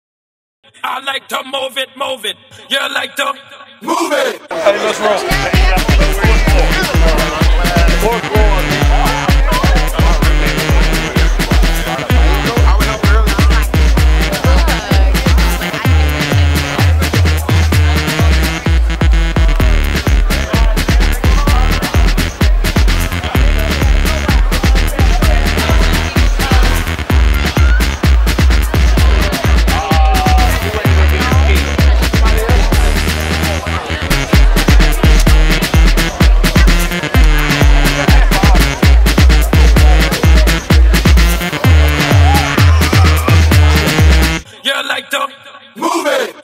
2015 South Alabama Football: "Move It"
Jaguar Football Team helping South Alabama students on 2015 Move-In Day!